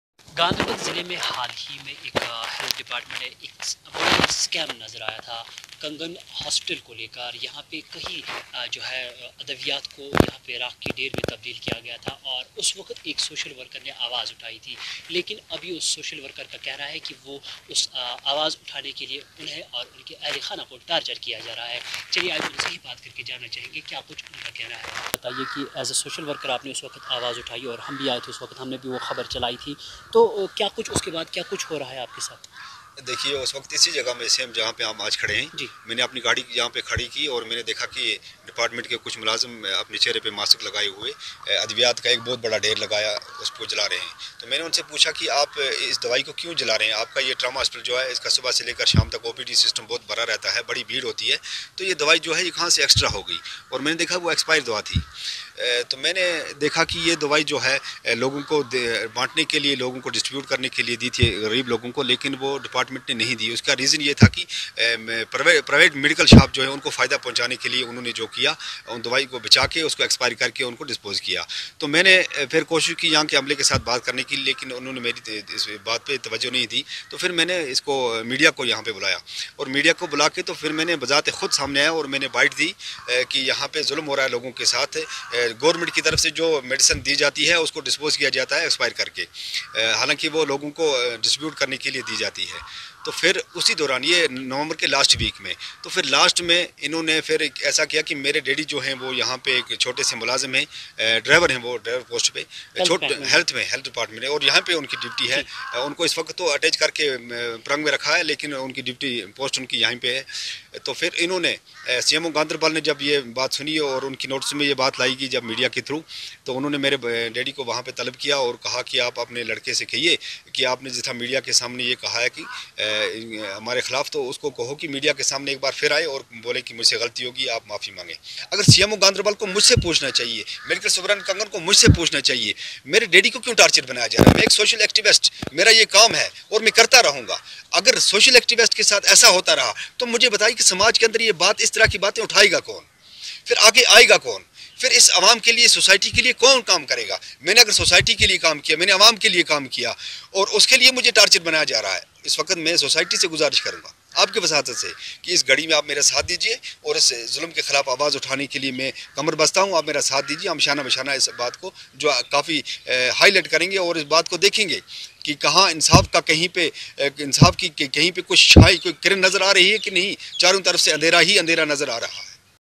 سماجی کارکن کو سچ بولنا مہنگا پڑ گیا
ریاست جموں و کشمیر کے ضلع گاندربل کے چھترگل علاقے سے تعلق رکھنے والے ایک سماجی کارکن چودھری عطا محمد نے محکمہ صحت پر الزام عائد کرتے ہوئے کہا کہ ان کا سچ بولنا ان کے اہل خانہ کے لیے پریشانی کا سبب بن رہا ہے۔

چودھری عطا محمد کے مطابق 4 مہینے پہلے ٹرامہ ہسپتال کنگن میں سینکڑوں روپے کی ادویات کو جلا کر راکھ بنا دیا، جبکہ محکمہ کے چند ملازمین چہروں پر ماسک لگا کر اس ادویات کو جلارہے تھے۔